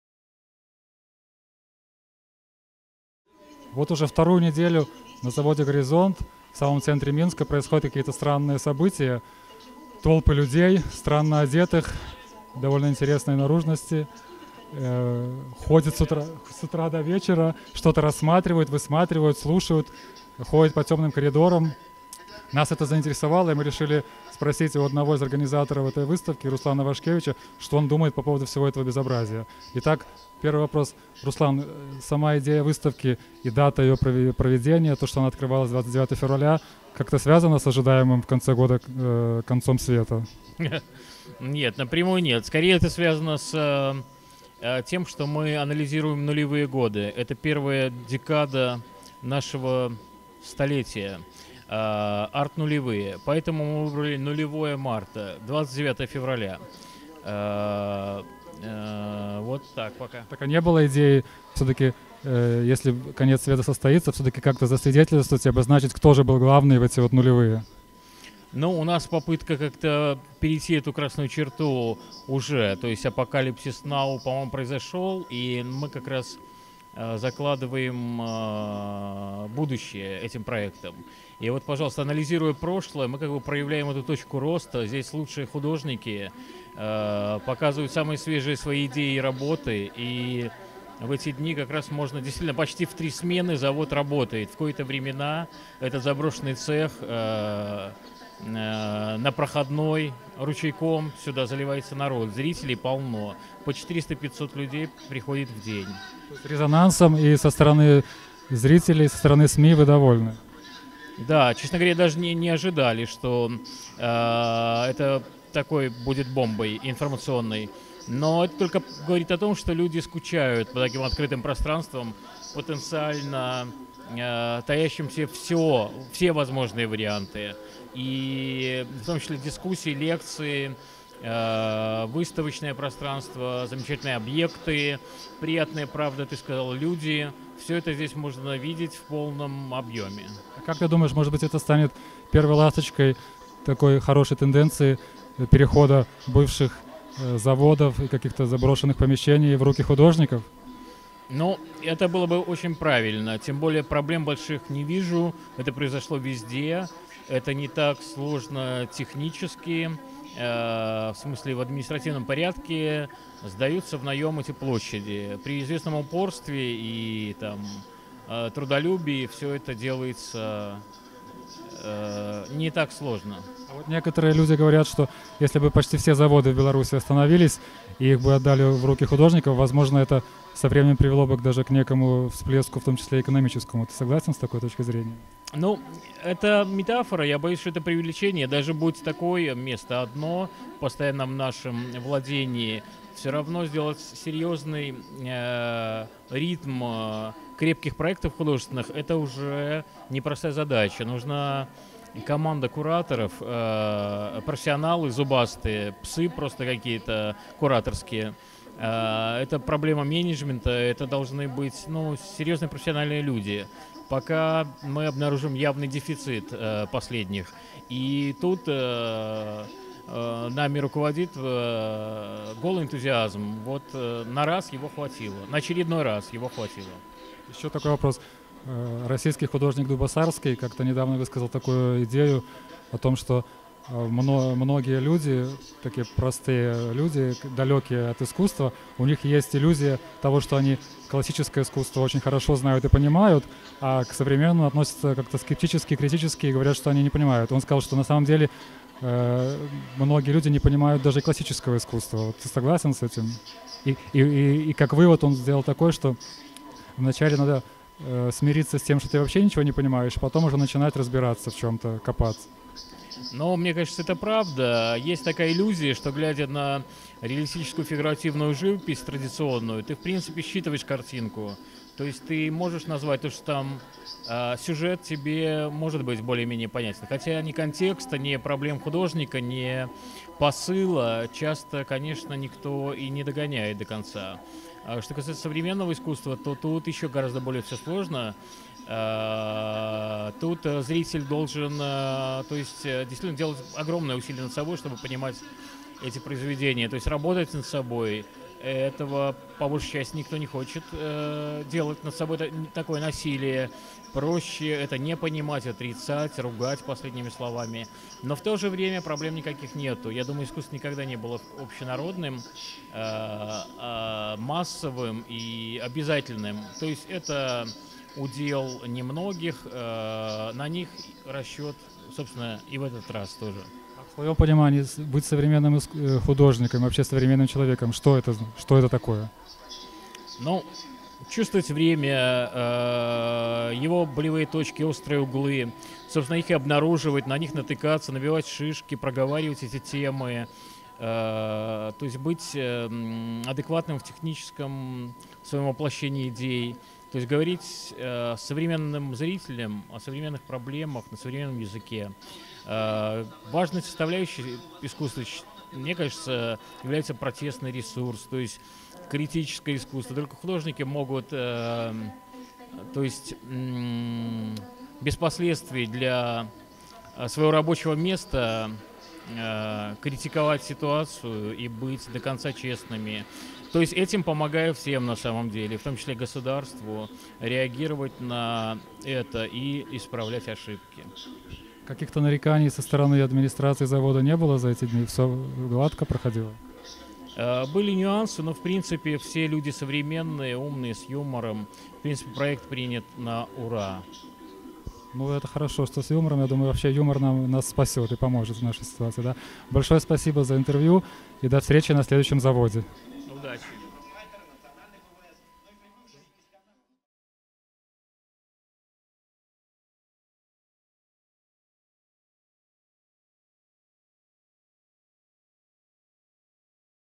Руслан Вашкевич. Выставка "Радиус нуля"
Проходившая с 29 февраля по 10 марта на заводе «Горизонт» выставка «Радиус нуля» засвидетельствовала, что какое-то искусство в Беларуси в новом веке все-таки было.

Кто-то даже  называет  его современным, но может ли оно быть таковым в несовременной стране? Теоретически  — да, но выставленное на всеобщее обозрение в центре Минска арт-наследие нулевых убеждает скорее в обратном. При этом я не разделяю мнения о том, что выставка провалилась, а ее организаторы и участники — это некие самозванцы, сами себя назначившие главными производителями образов в этой стране. Это на самом деле (почти) лучшее из того, что мы можем предъявить миру, жаль только, что мир видел все это 50 лет назад.

Главной проблемой нашего общества является  не мифическая диктатура, которая душит все творческие начинания «элиты», а неамбициозность , необразованность и неактуальность тех, кого, собственно, надо «душить».  Не профессиональные борцы с «режимом» нам нужны и не инфантильные абсольвенты ЕГУ с провинциальным мышлением,  а «мастера иллюзий с тонким пониманием духа времени», которые бы внятно сформулировали и донесли до общества  понятия «красоты» и «ценности». Пора объявить войну простоте, инстинктам и житейской мудрости. Человек, который перестает развиваться и усложняться — деградирует. У людей, мир которых равен самому себе, нет будущего.

А пока нам не остается ничего другого, кроме как бросать в наше национальное болотце камушки, или бетонные плиты, как это сделали Руслан Вашкевич, Ольга Шпарага и Оксана Жгировская. Вопрос в том, есть ли у нас несколько десятков лет для того, чтобы почувствовать под ногами дно.